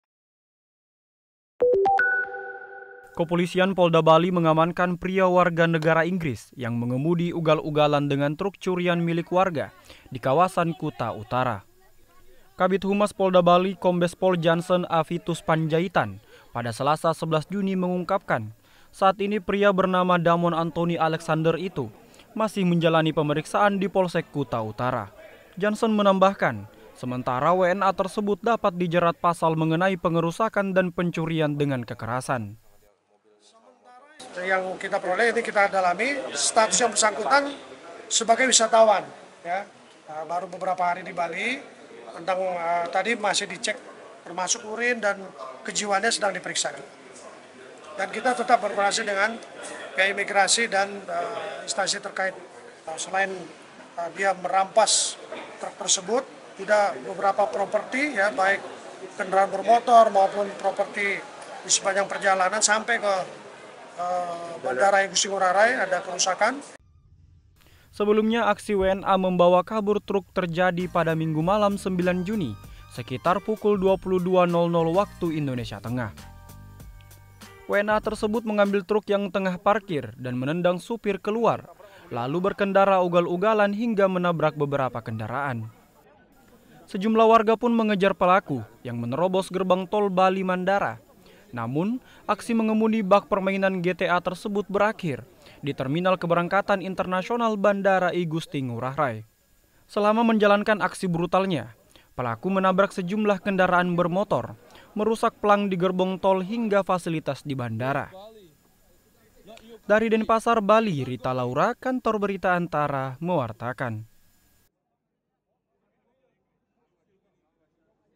Kepolisian dalami kejiwaan WNA yang ugal-ugalan dengan truk curian
ANTARA - Kepolisian di wilayah Polda Bali memeriksa kejiwaan WNA asal Inggris yang mengemudi ugal-ugalan dengan truk curian milik warga. Kabid Humas Polda Bali Kombes Pol Jansen Avitus Panjaitan di Denpasar, Selasa (11/6), mengungkapkan WNA tersebut merupakan wisatawan yang baru tiba beberapa hari di Bali. (Rita Laura/Satrio Giri Marwanto/Farah Khadija)